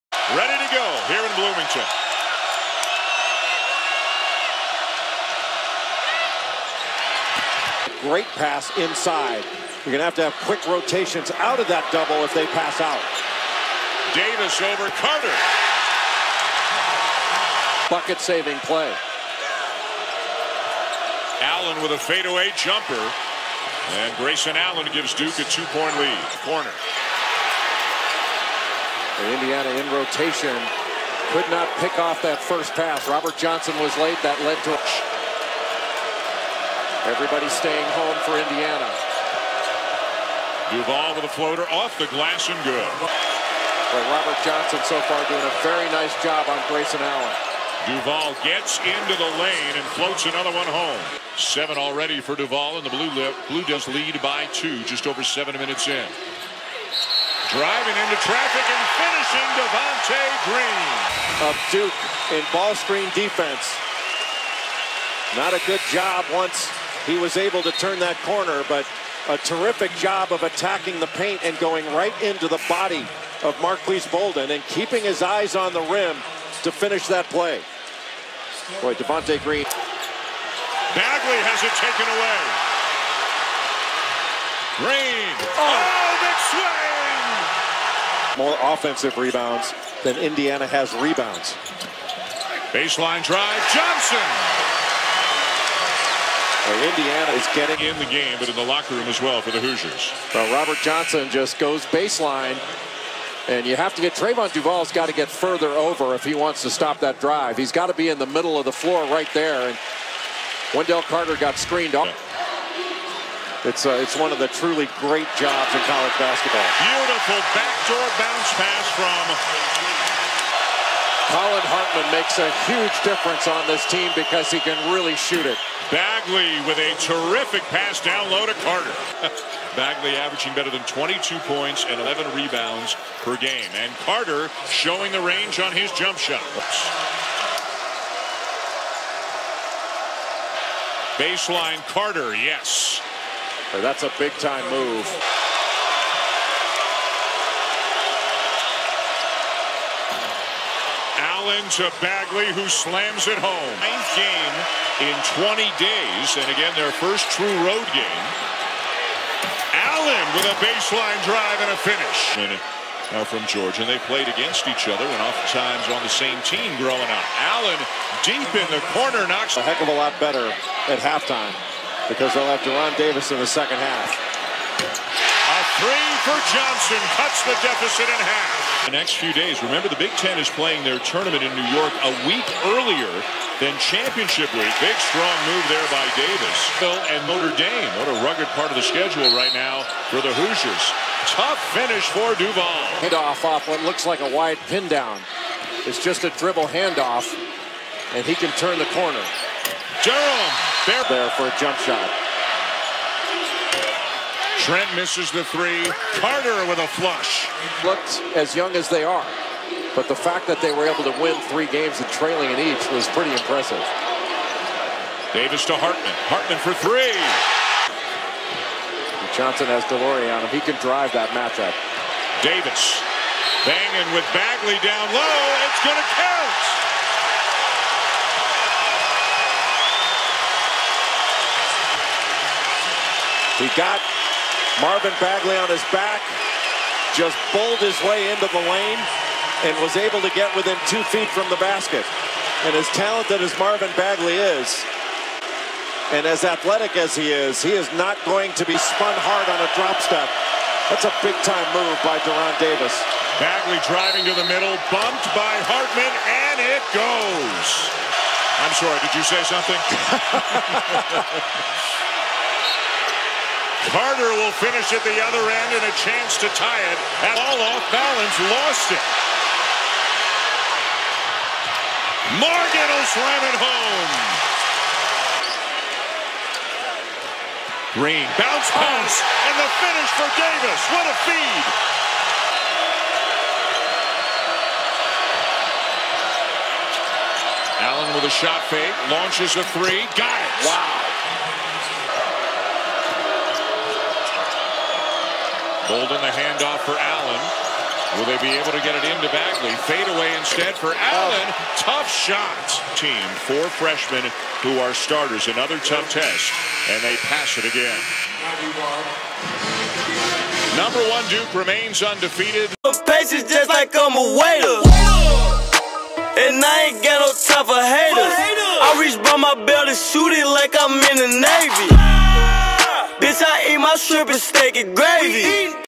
[ACC/BIG TEN CHALLENGE] Duke vs Indiana_GameHighlight|2017.11.30
IU scored with No. 1 Duke for 35 minutes Wednesday — and battled for 40 — falling 91-81 to the Blue Devils in the ACC/Big Ten Challenge.